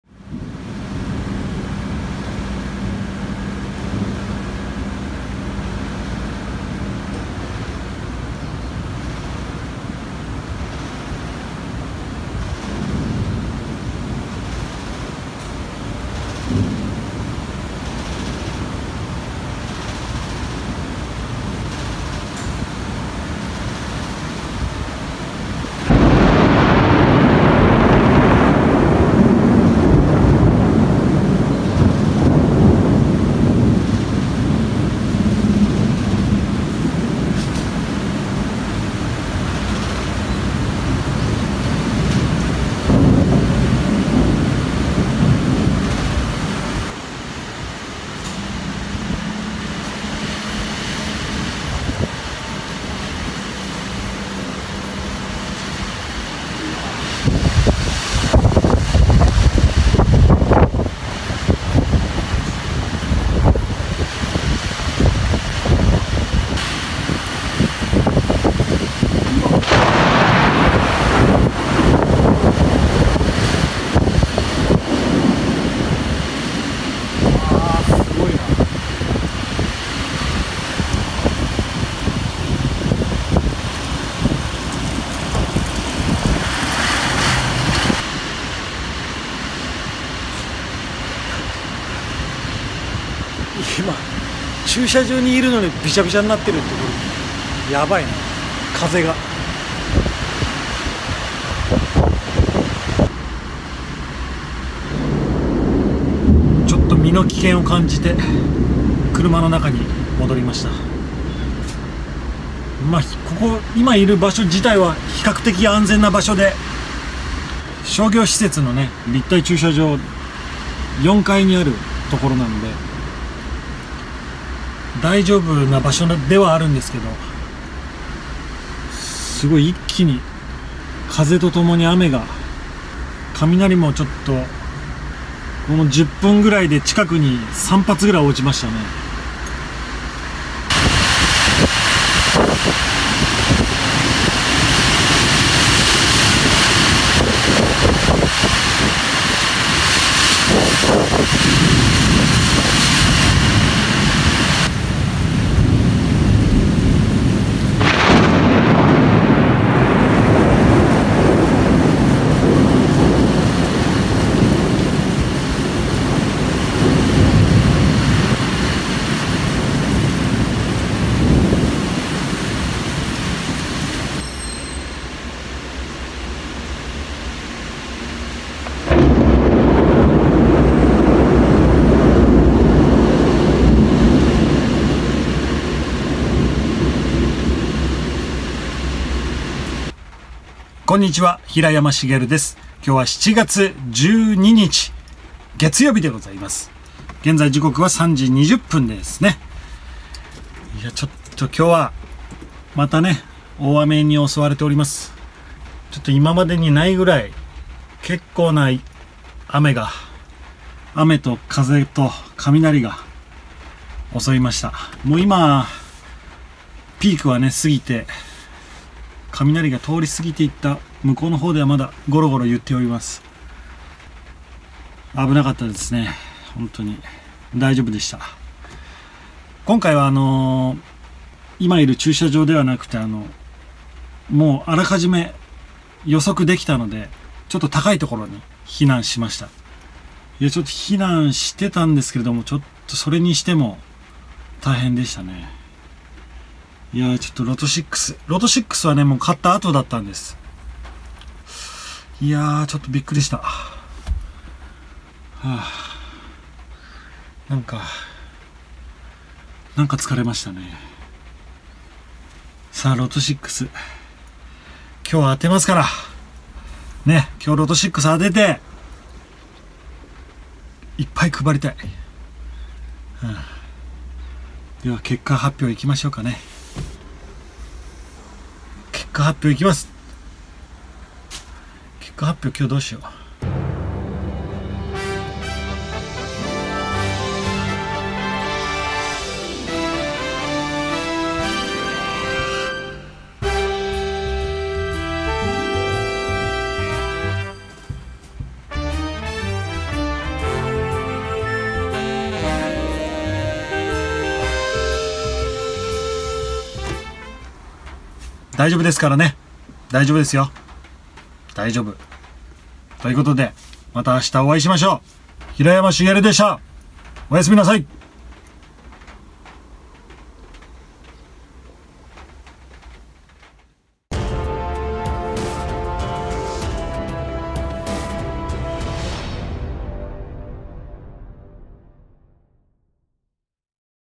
【月曜日企画】ロト6結果発表！ の前に襲われました。大雨落雷強風 2021年7月12日(月) 大丈夫だよシリーズ
毎週月曜日はロト6 企画をしています。
高額当選したらあなたにプレゼント発動します！
金銭的にも大丈夫に！